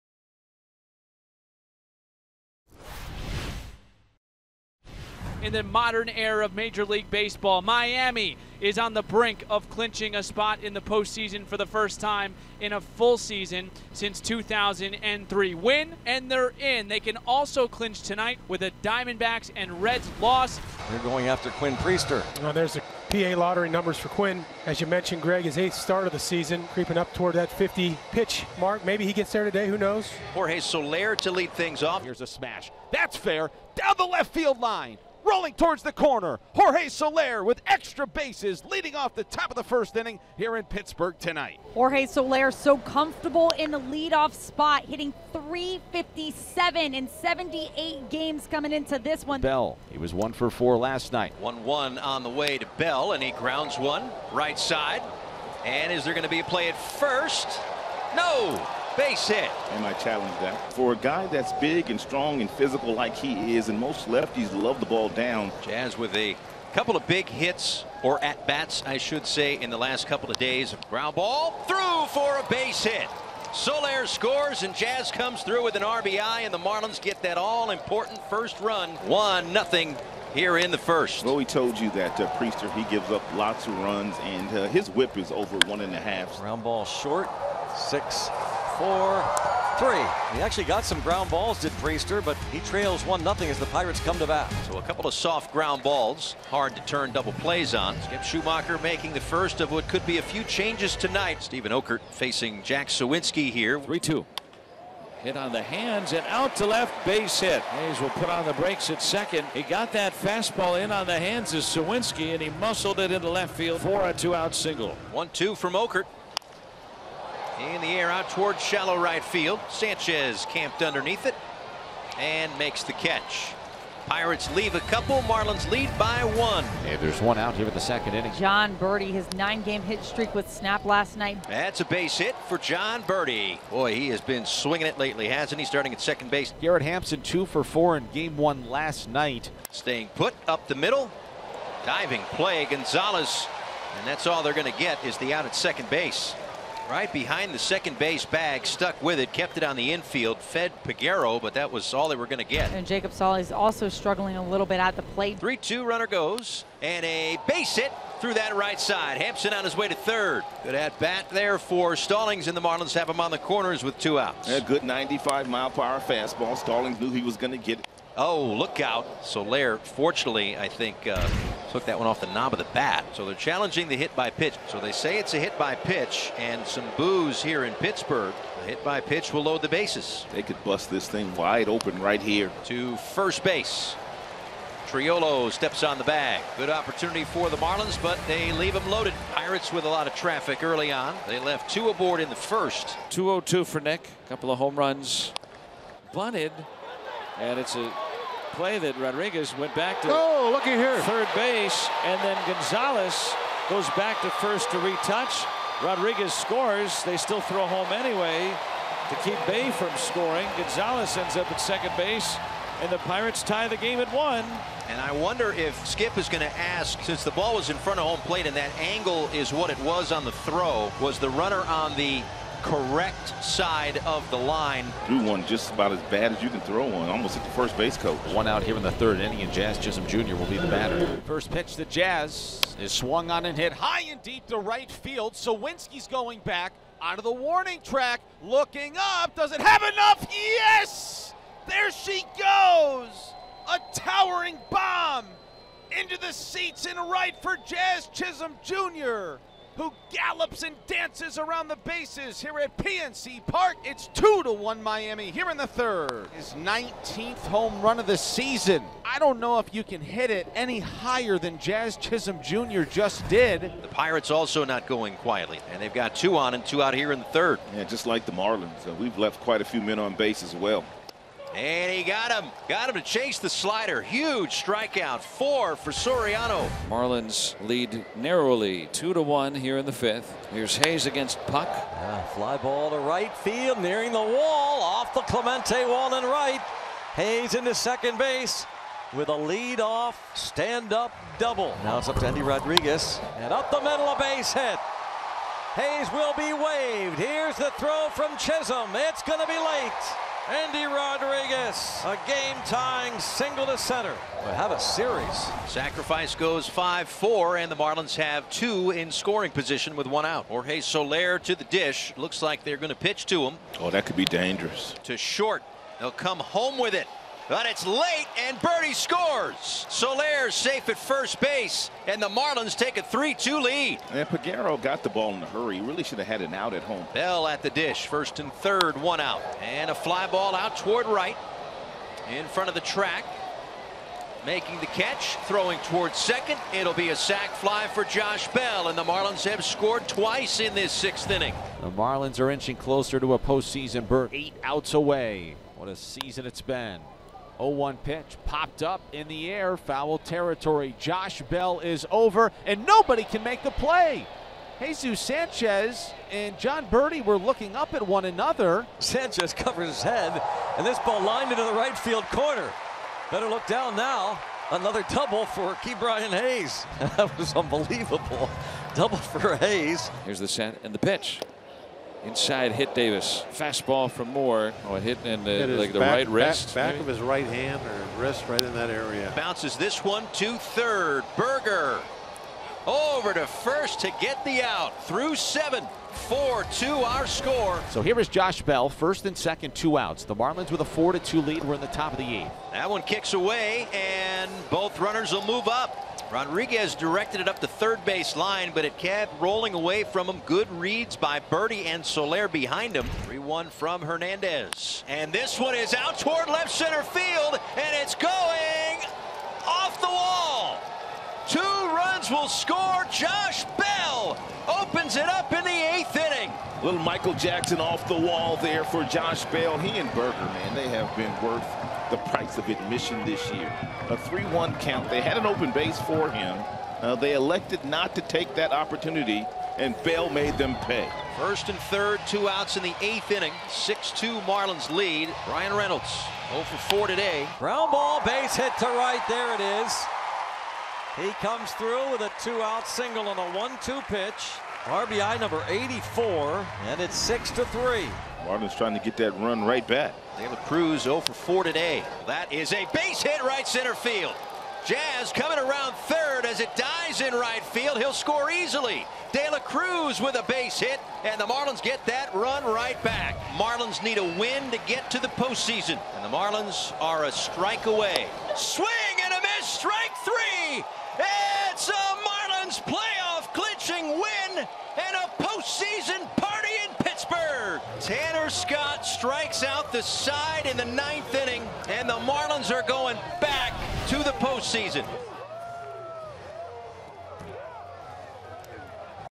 Marlins vs. Pirates Game Highlights (9/30/23) | MLB Highlights
Marlins vs. Pirates full game highlights from 9/30/23, presented by @helloRecreate

(1:06) Jazz Chisholm Jr. singles on a ground ball to right field. One run scores.
(3:11) Nick Gonzales hits a sacrifice bunt. One run scores.
(3:46) Jazz Chisholm Jr. hits to solo home run
(5:10) Jorge Soler reaches on a fielder's choice. One run scores.
(5:26) Josh Bell out on a sacrifice fly to right field. One run scores.
(6:41) Josh Bell doubles on a fly ball to center field. Two runs score.
(7:14) Bryan Reynolds singles on a ground ball to right field. One run scores.
(7:29) Bryan De La Cruz singles on a ground ball to right field. One run scores.